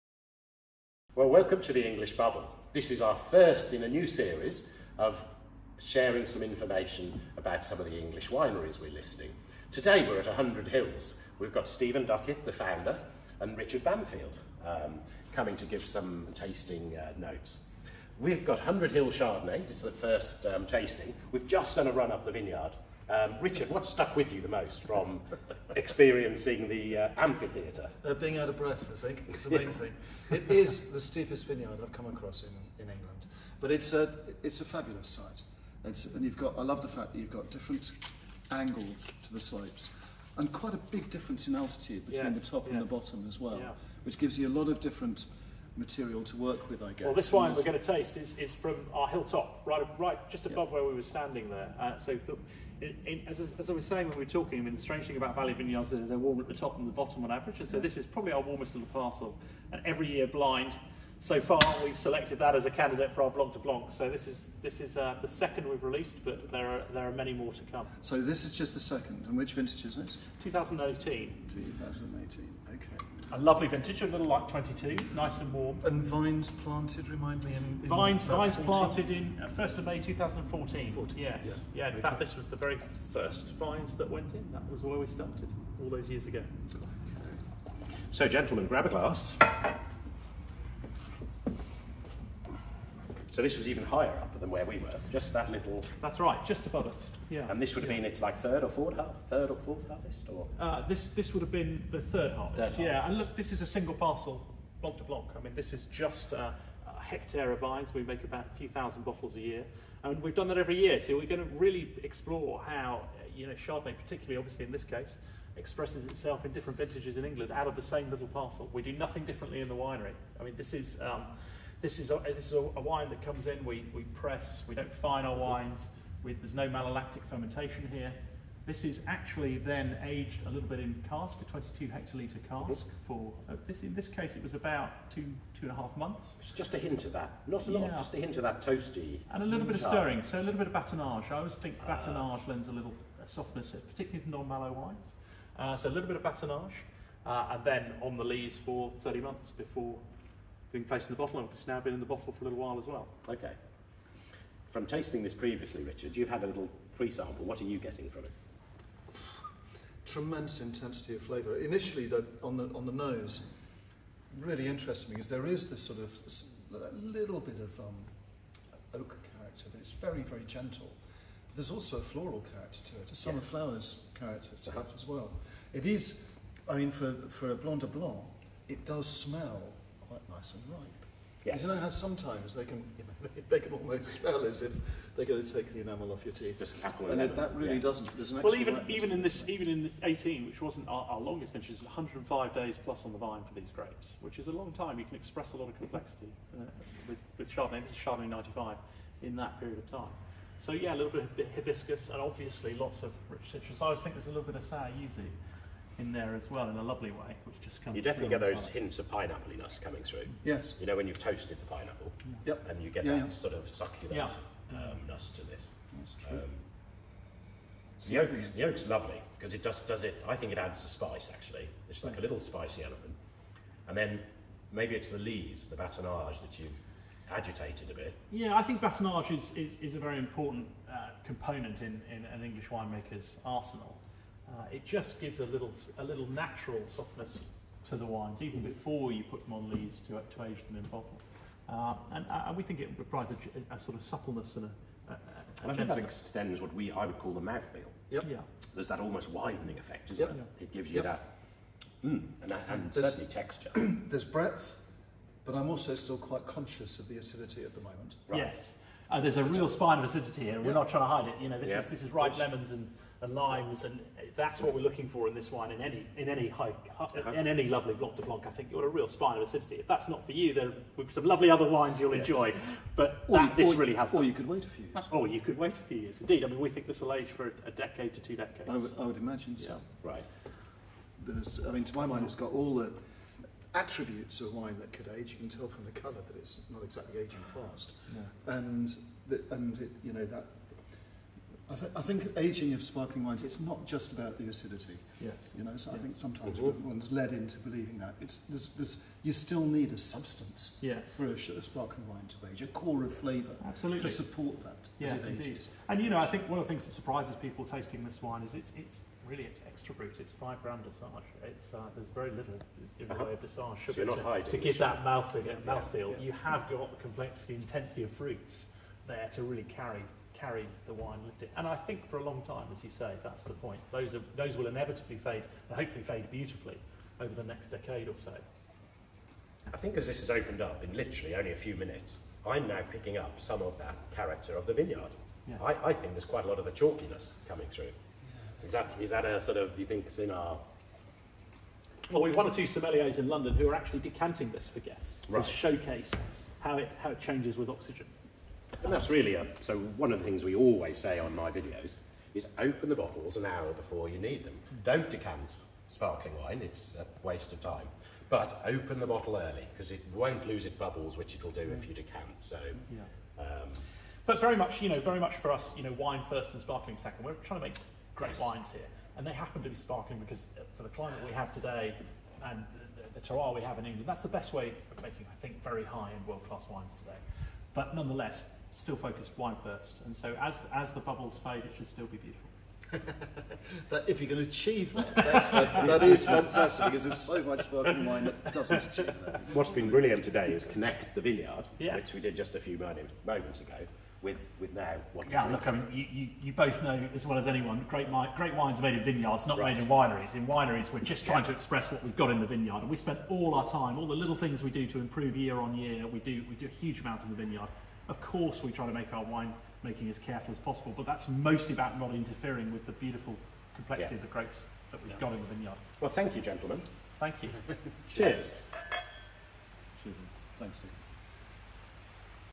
Founder of Hundred Hills Stephen Duckett, Richard Bampfield and I discuss their Blanc de Blancs 2018
Part 2 of 3 following our visit to the English Sparkling Range winery Hundred Hills

Founder Stephen Duckett, Richard Bampfield and I discuss Hundred Hills Blanc de Blancs 2018 having had a tour of the vineyard in part 1